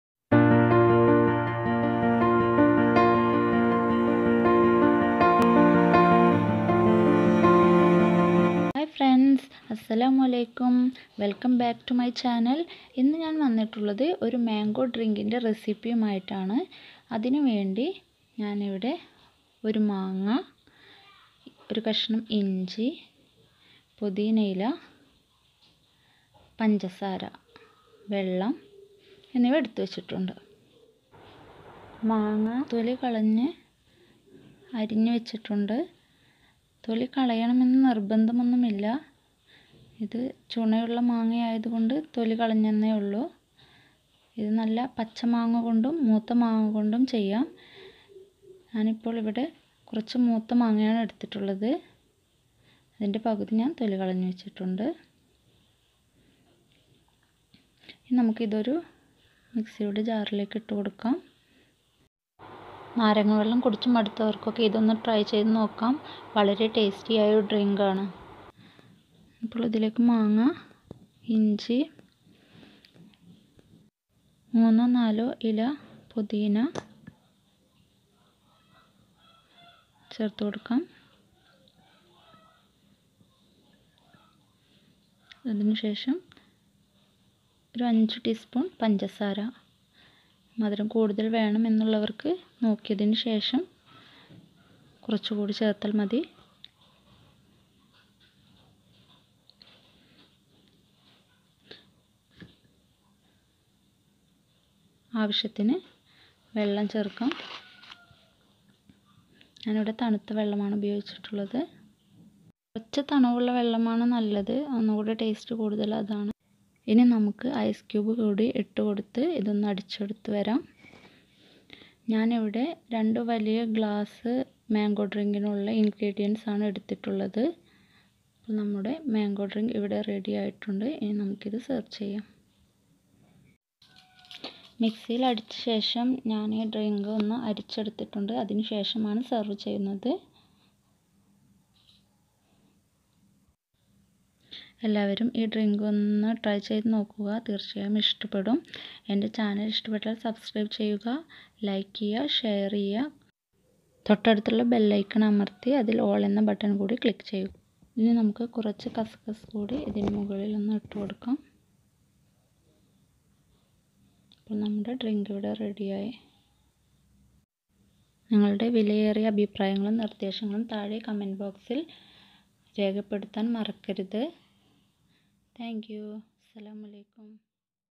|Mango| Drink|niyoos world|
INCREADIANTS
Mango
Ginger
Mint leaves
Sugar
Water